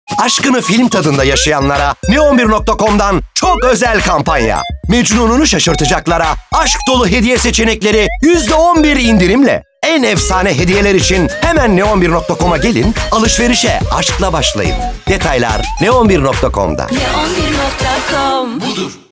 Sevgililer Günü - Mecnun'unu Şaşırtacaklara!
Bir kadına hediye almak ne kadar kolaysa erkeğe hediye almak da o kadar zordur derler. Kadınlar için alınabilecek hediyeler arasında tekstilden çanta ve ayakkabıya, takıdan aksesuara, makyaj malzemelerinden şık iç çamaşırına kadar çeşit çeşit hediye seçeneği mevcut. Öte yandan bir erkek için alınabilecek hediyeler maalesef kadınlara oranla daha kısıtlı ve az çeşitli kalabiliyor. Siz de erkek sevgiliye hediye konusunda bu dertten muzdaripseniz, aradığınız hediyeyi burada bulmanız kuvvetle muhtemel. Gelin erkek sevgiliye hediye fikirleri arasından önerilerimize birlikte bakalım